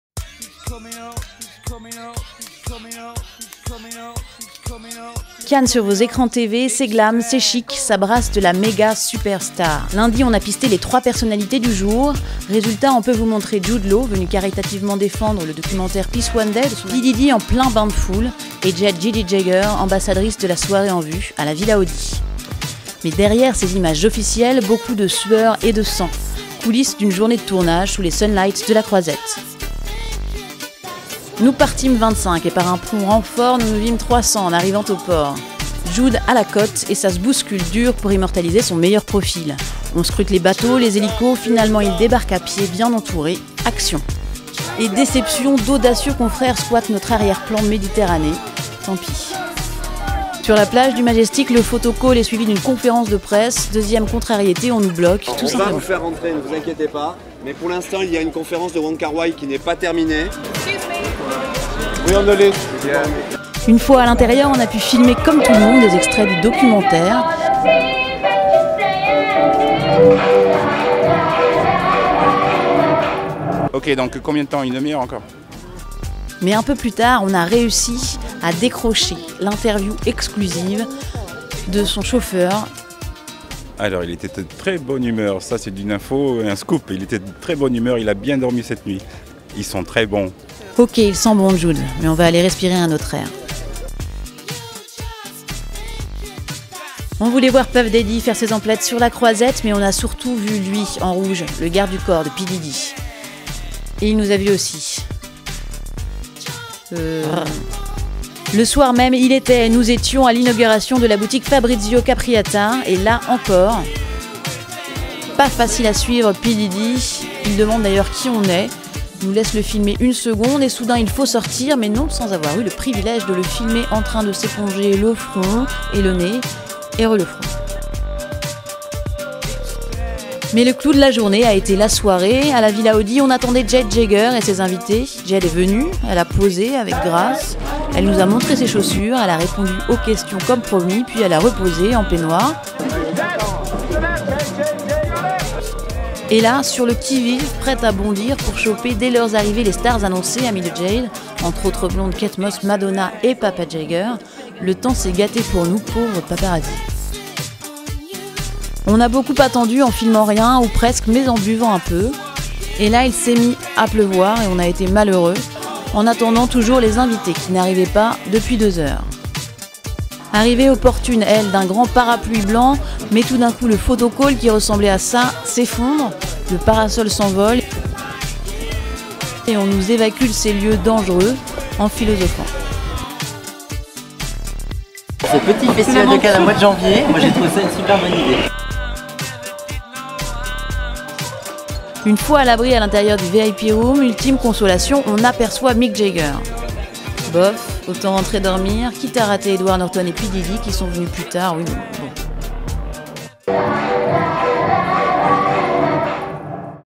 Jude Law, P. Diddy et Jade Jagger au festival de Cannes 2008 - Gala
Cannes sur vos écrans TV, c'est glam', c'est chic, ça brasse de la méga superstar. Lundi, on a pisté les trois personnalités du jour. Résultat, on peut vous montrer Jude Law, venu caritativement défendre le documentaire Peace One Day, P. Diddy en plein bain de foule, et Jade Jagger, ambassadrice de la soirée en vue à la villa Audi...